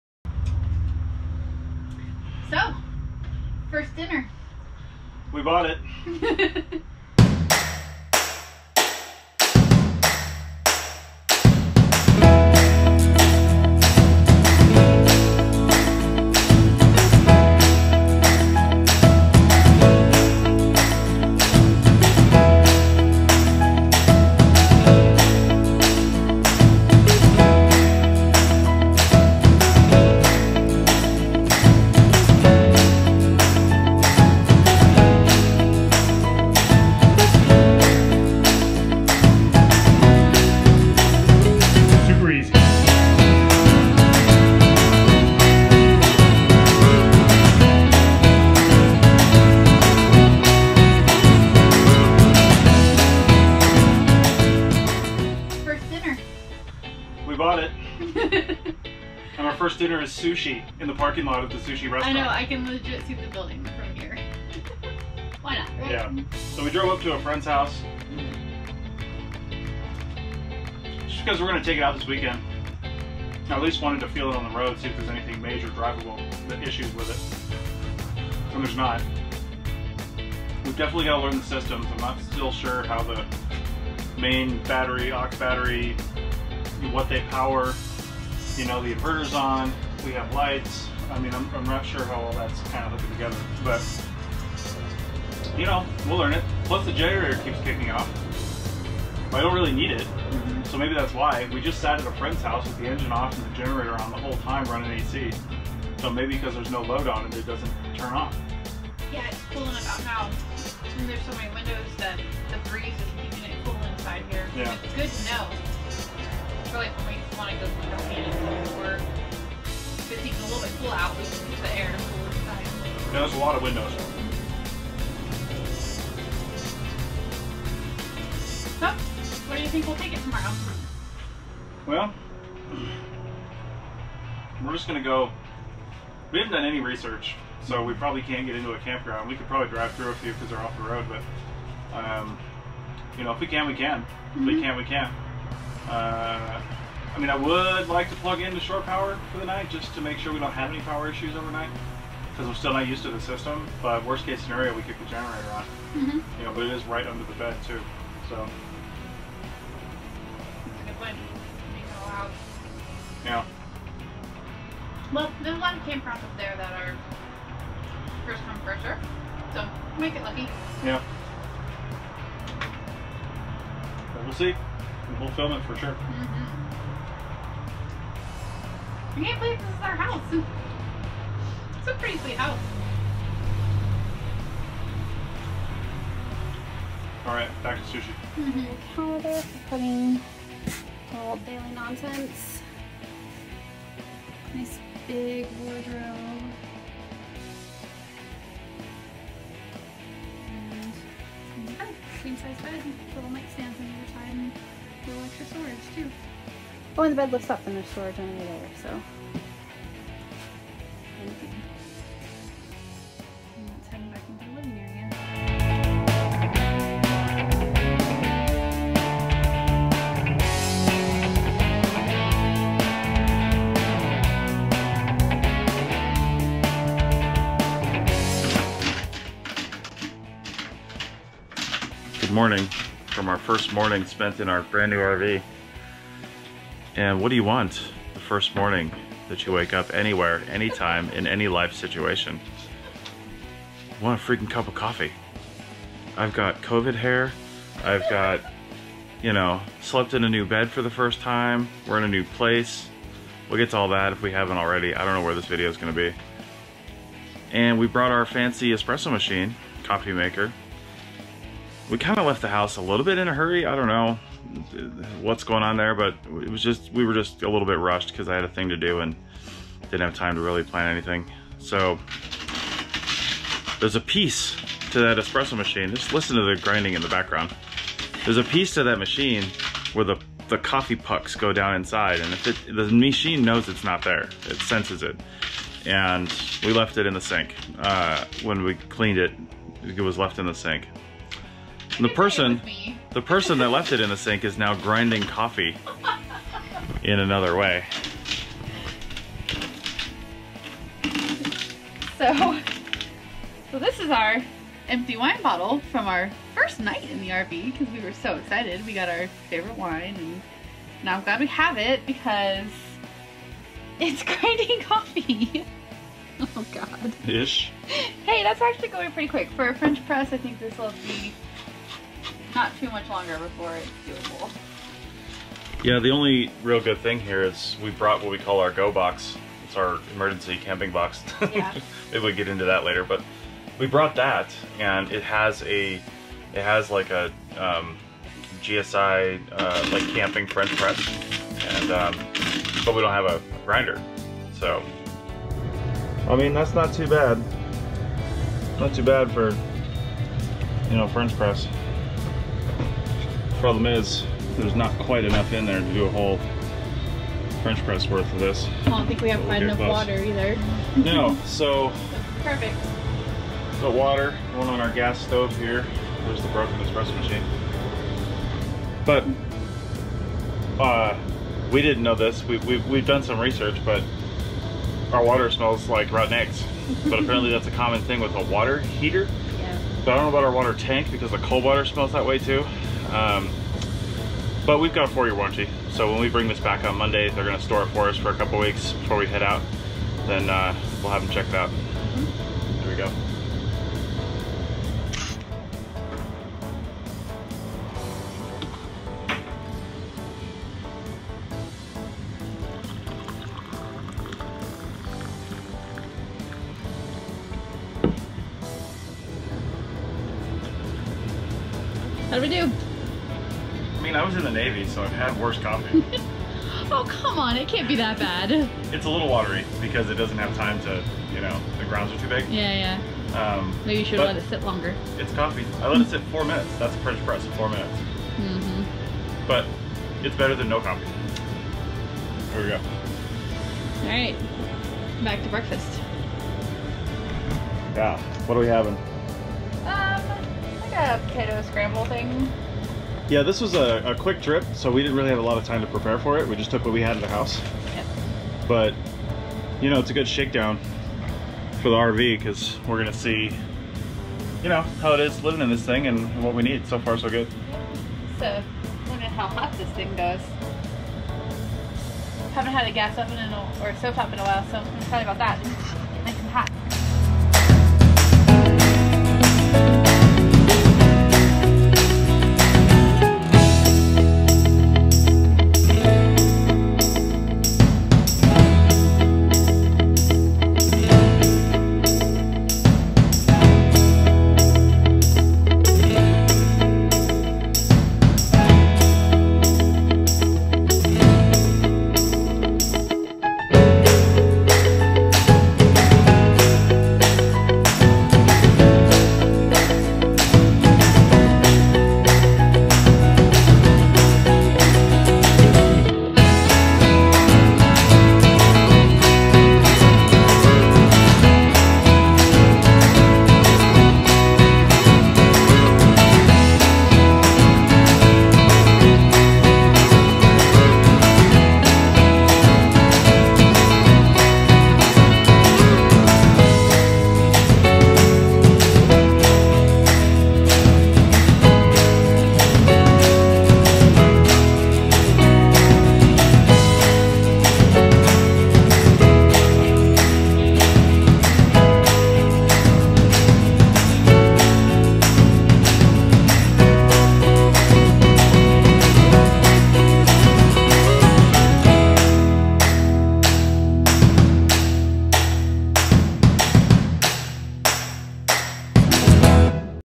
Holiday Rambler Vacationers - Ep 17
We bought an RV! Our first RV is a 2015 36ft Holiday Rambler Vacationer SB. In this video you'll see our first morning, first dinner, first drive, and first park! There are more detailed walk-throughs coming but this is a first start.